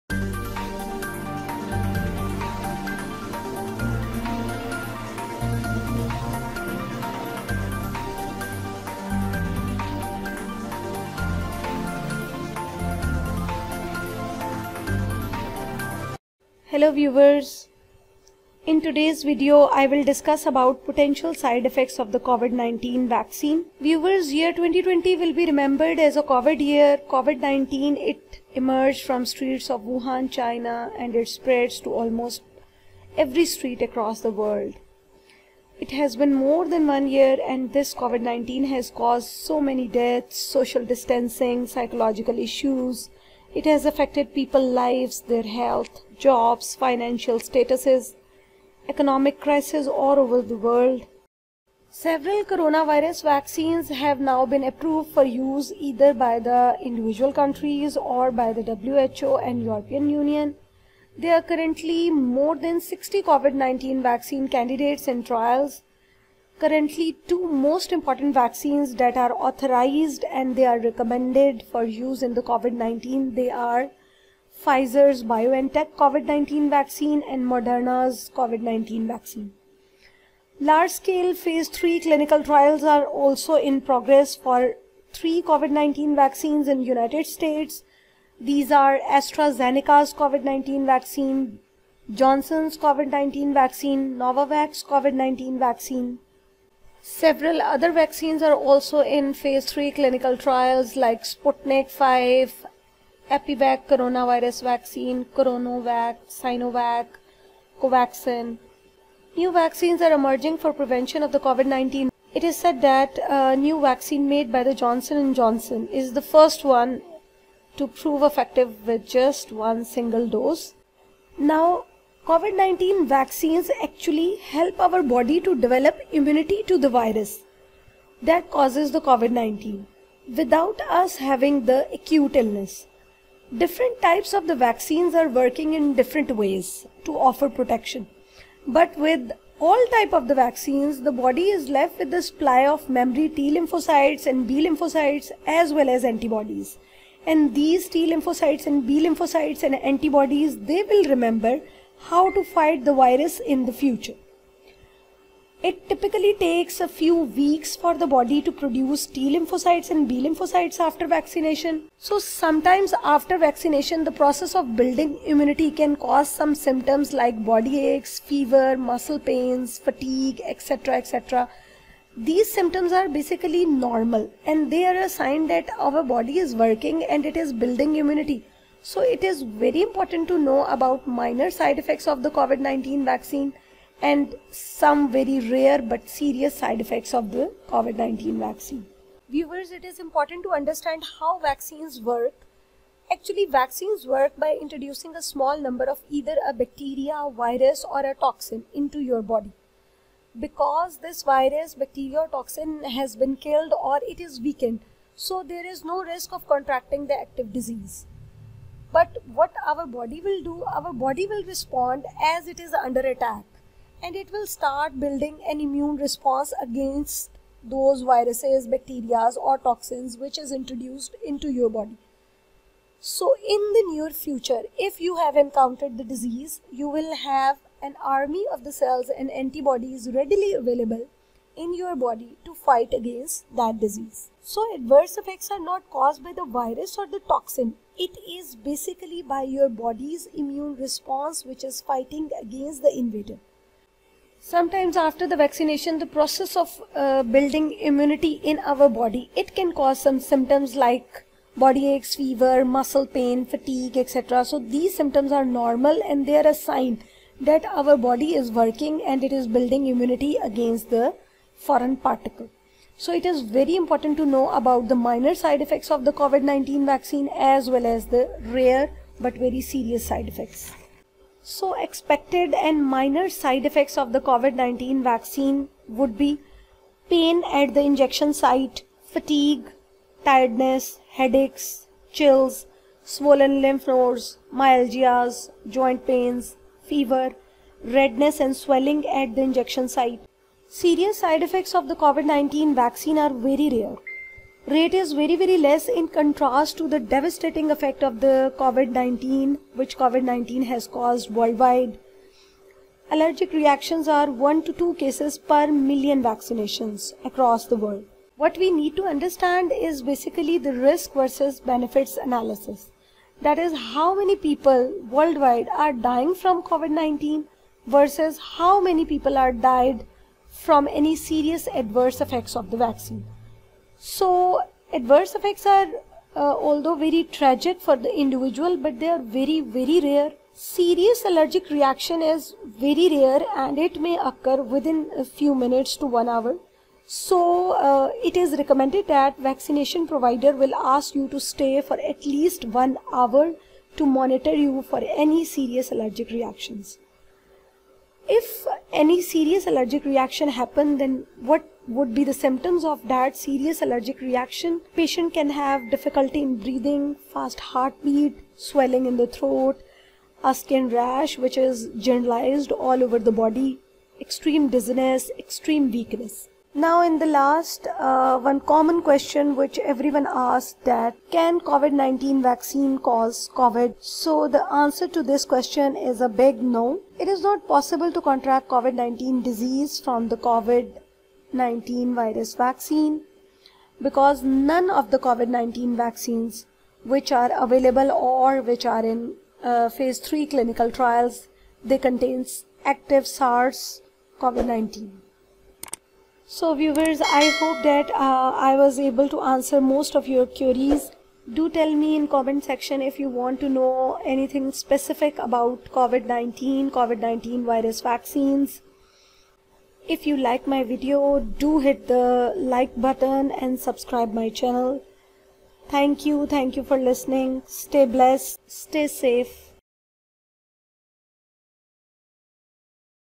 Covid 19 vaccines | Side Effects of Covid 19 Vaccine | Efficacy and safety facts with Pulmonologist
As the vaccine to protect against Covid-19 continues to roll out to more and more people, interest in side effects is high. This can lead to lots of media coverage when people experience side effects, particularly when serious side effects occur. Consultant Pulmonologist Dr. Sidra Razzaq talks about side effects and put some of these stories in context, which will hopefully leave less room for disiformation.